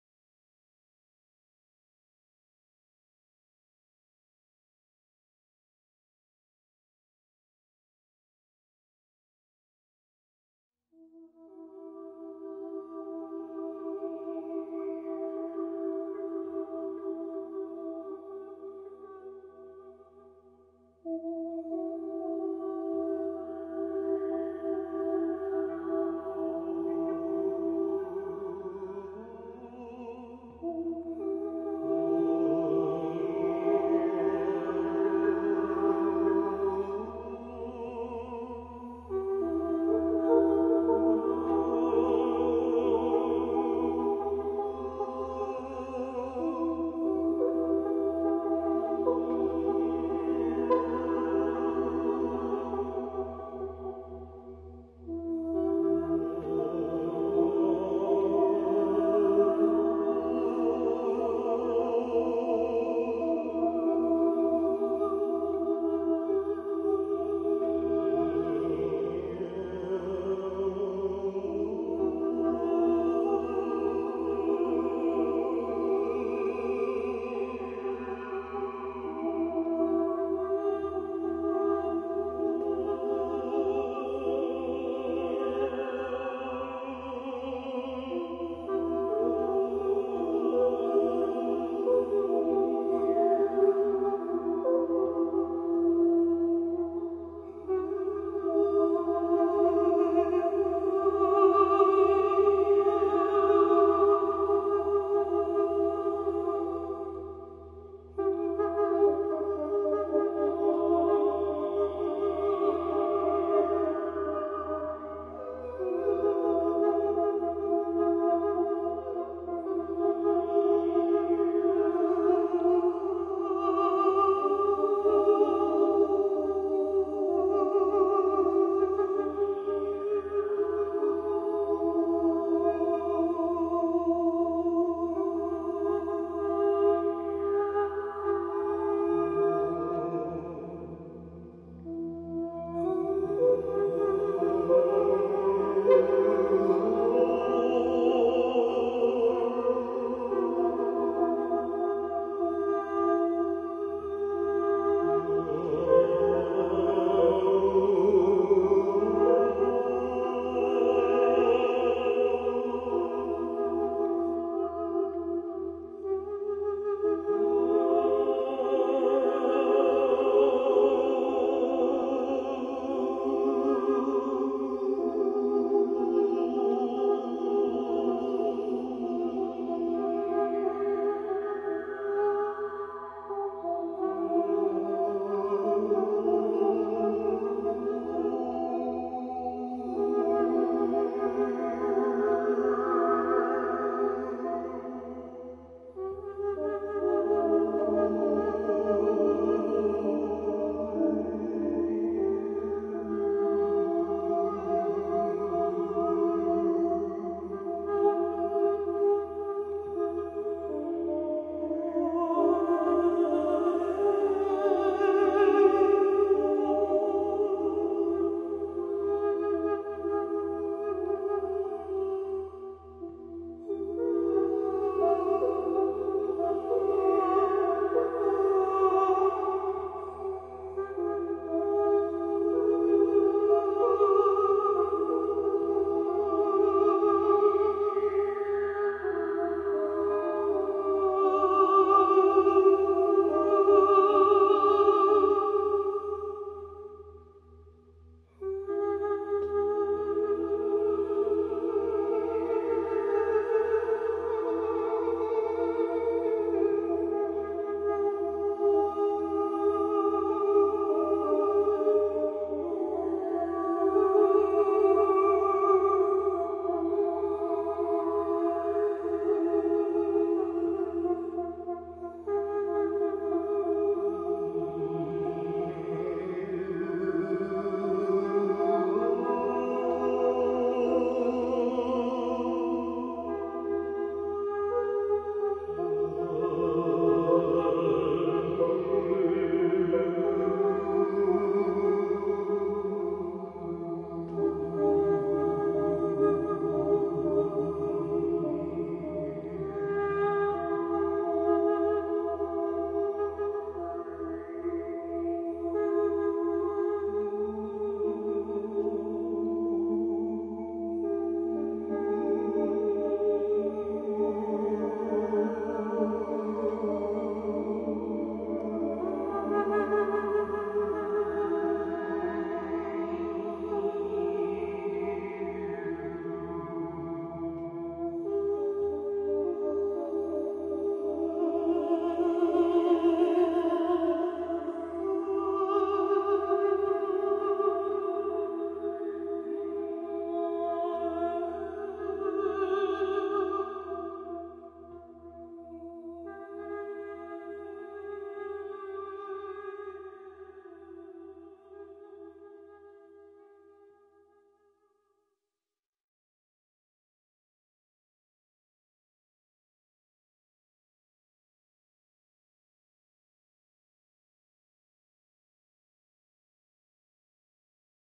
Glenn Shamandrum - part 1
making a frame for a Shamandrum and listen to the song it's good for your third eye